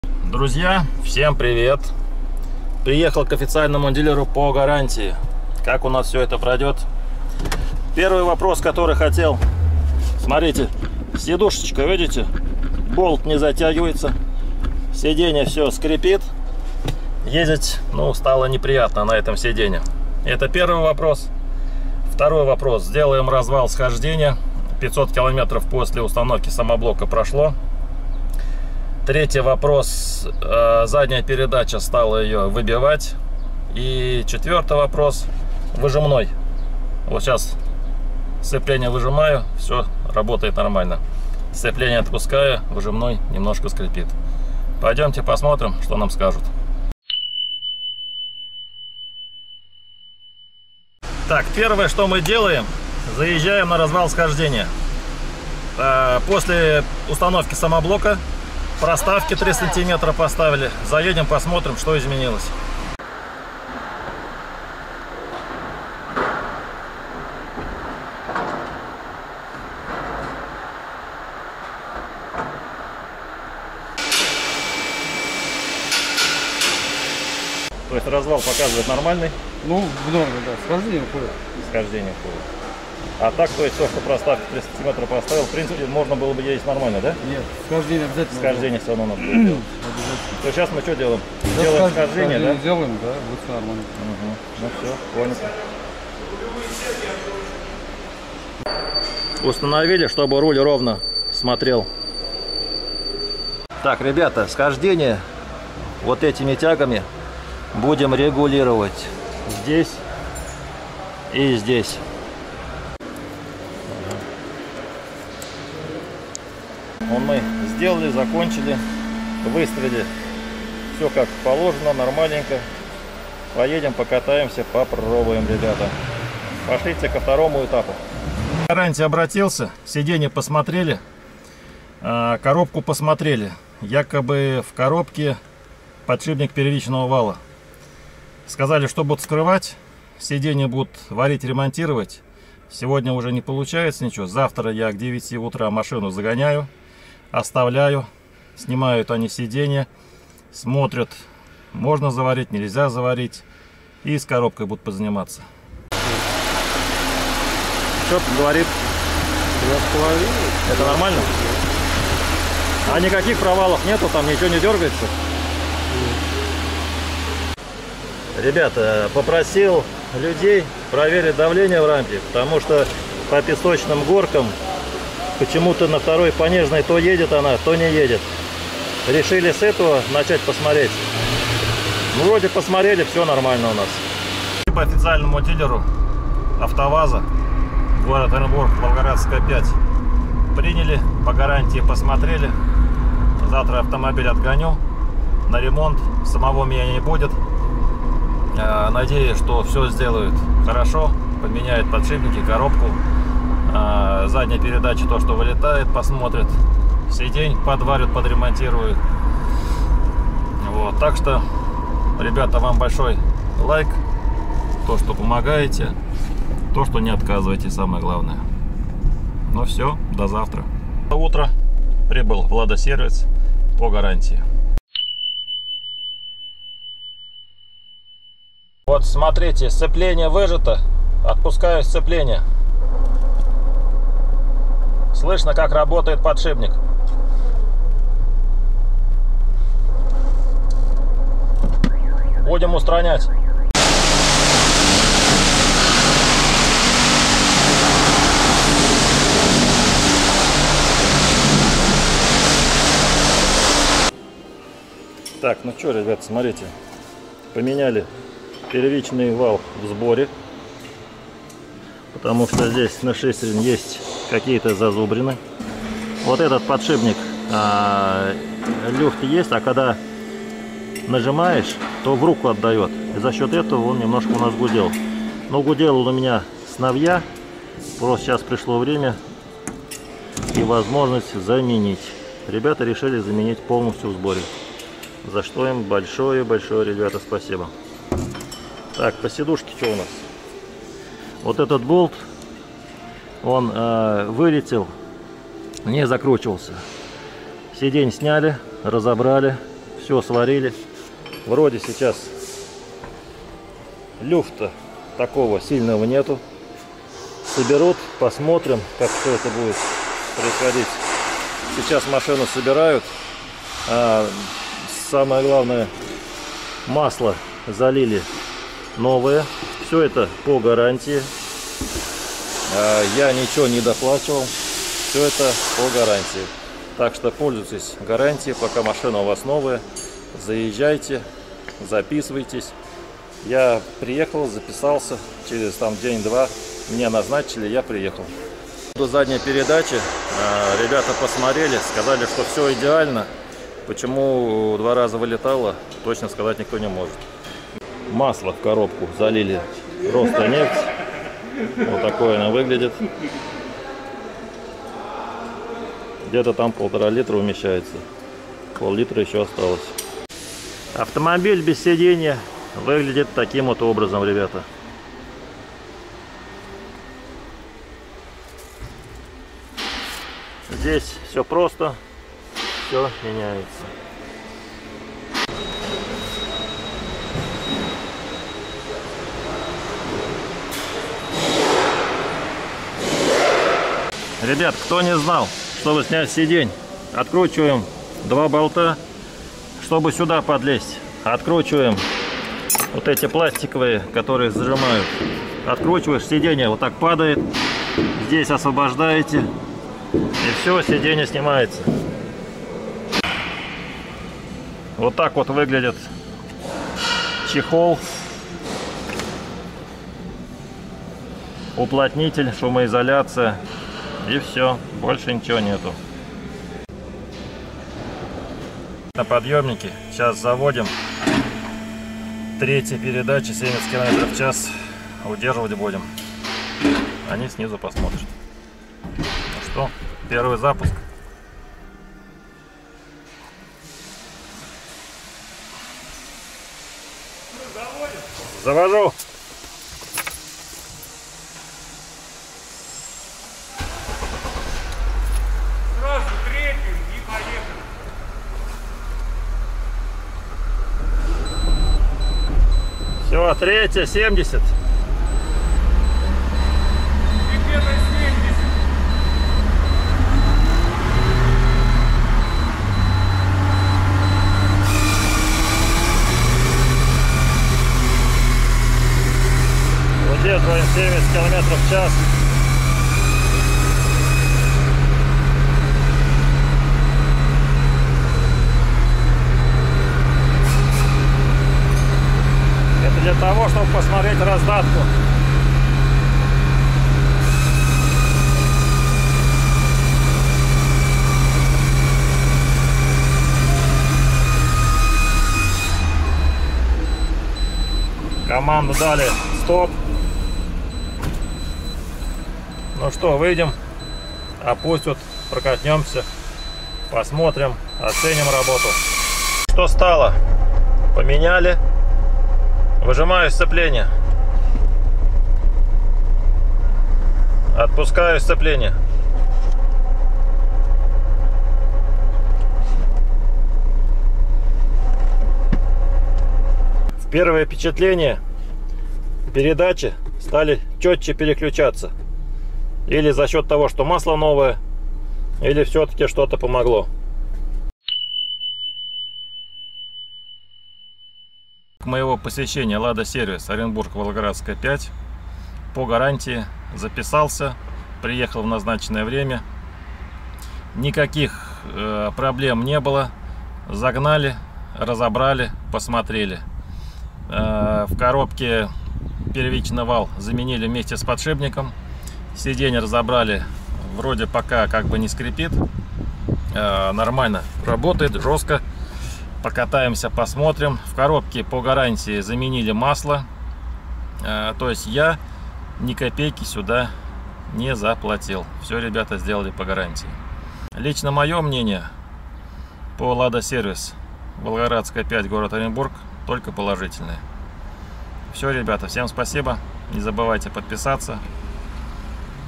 🔥ГАРАНТИЯ ЛАДА В ДЕЙСТВИИ🔥/ЛАДА-СЕРВИС ОРЕНБУРГ🔥
1. ГАРАНТИЯ ЛАДА В ДЕЙСТВИИ. РАЗВАЛ СХОЖДЕНИЯ СТОИЛ МНЕ 900 Р.
2.  НА РАЗВИТИЕ КАНАЛА : СБЕРБАНК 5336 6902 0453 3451 ВИТАЛИЙ СЕРГЕЕВИЧ О